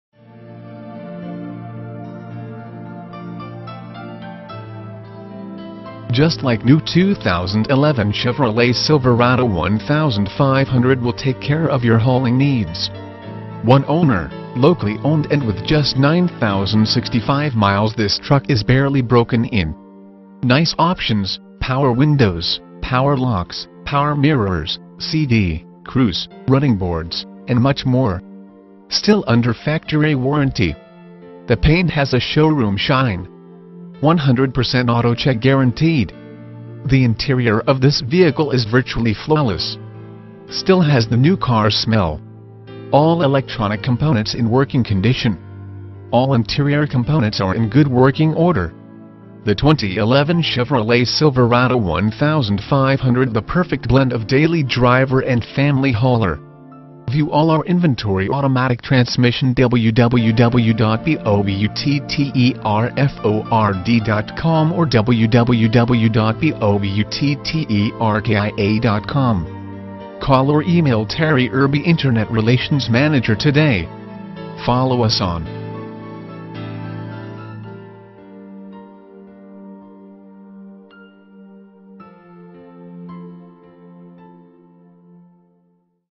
2011 Chevrolet Silverado 1500 - Blake Utter Ford
215 N. Hwy 75

For Sale: 2011 Chevrolet Silverado 1500
VIN: 1GCRCREA9BZ180517
Engine: Vortec 4.8L Variable Valve Timing V8 SFI FlexFuel
Drivetrain:
Transmission: Automatic
Mileage: 9,065
Color: Summit White (exterior) Dark Titanium (interior)

Features:
Just like new 2011 Chevrolet Silverado 1500 will take care of your hauling needs!  One owner, locally owned and with just 9065 miles this truck is barely broken in!  Nice options: power windows, power locks, power mirrors, cd, cruise, running boards, and much more!  Still under factory warranty!  The paint has a showroom shine. 100% AutoCheck guaranteed! The interior of this vehicle is virtually flawless. Still has the new car smell! All electronic components in working condition. All interior components are in good working order. The 2011 Chevrolet Silverado 1500: the perfect blend of daily driver and family hauler.  View all our inventory automatic transmission